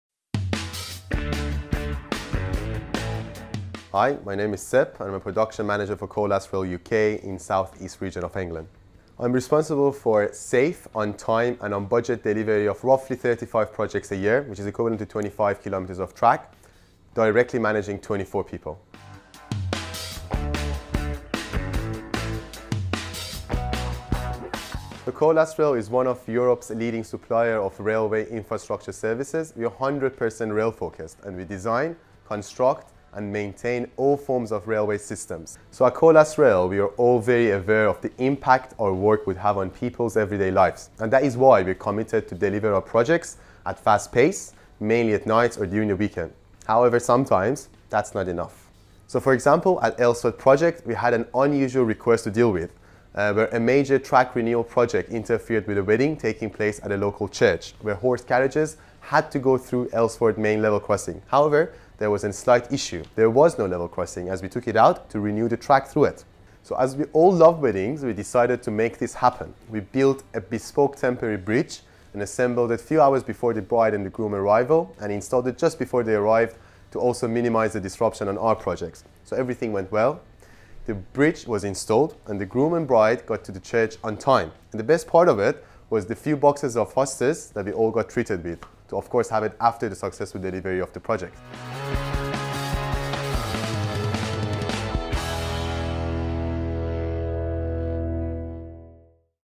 Sep's myjobstory
Sepehr Semsarzadeh's job story and find out how we got the bride and groom to the church on time!